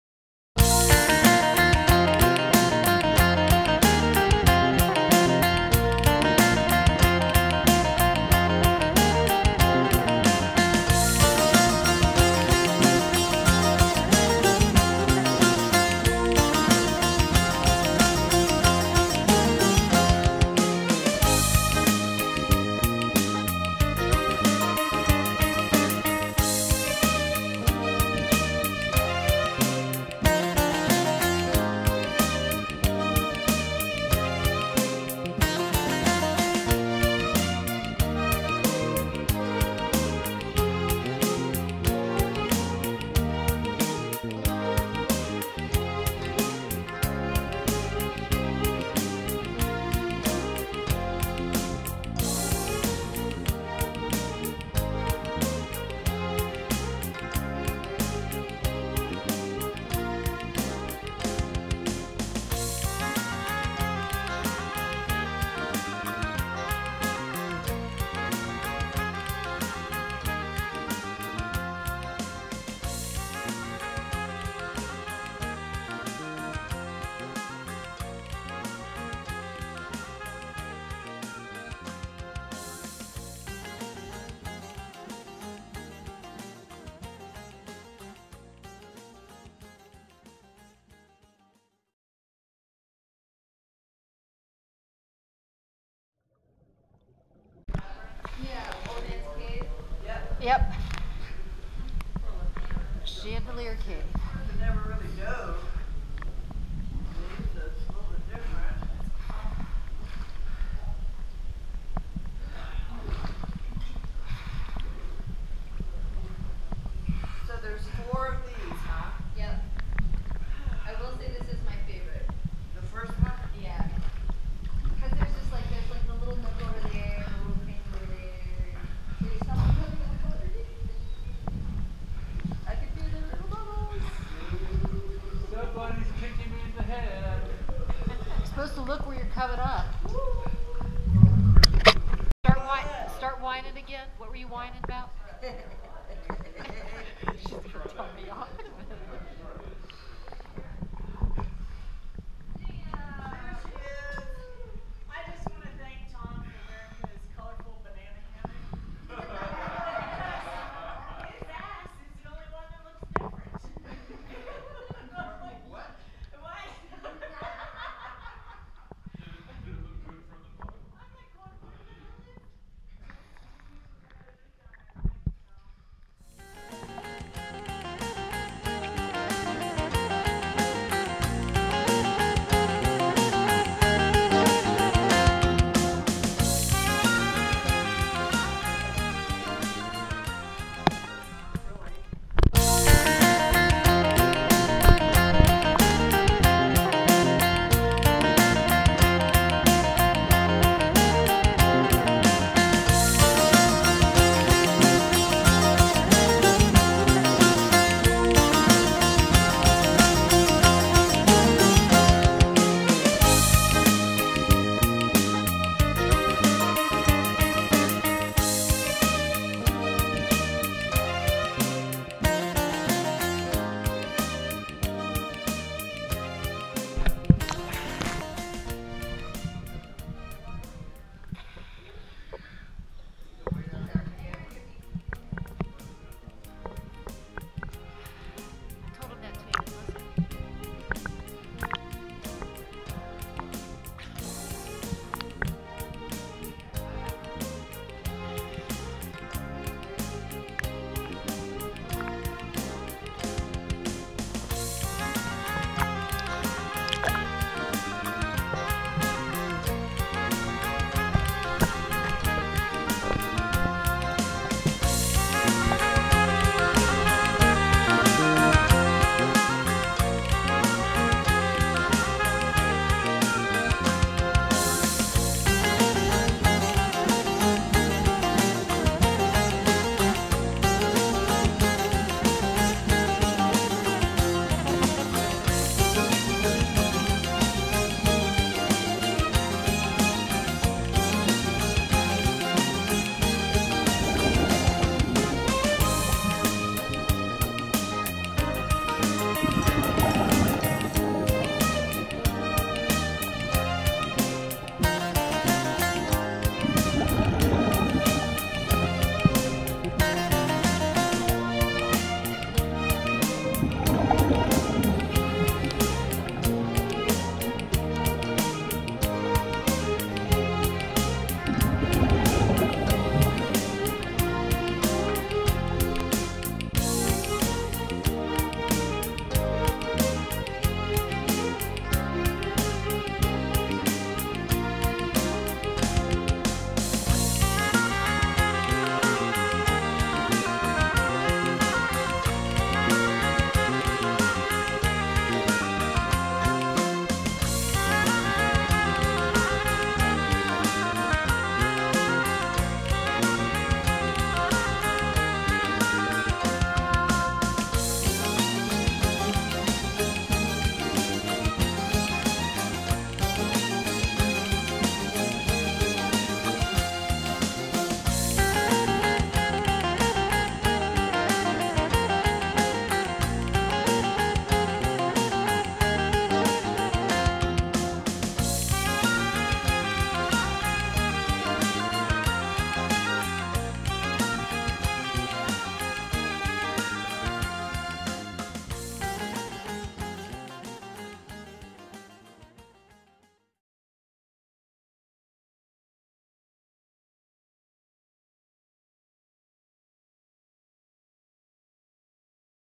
Chandelier Cave
Another famous dive in Palau. We did this dive mid afternoon. Cave is more like a serious of swim throughs that lead to open air caves. Watch as the stalactites pierce the calm clear surface darting into view. I remember entering 3 out of the 4 caves. Enjoy this video known to the local Palauans as " Cave of the Grouper ".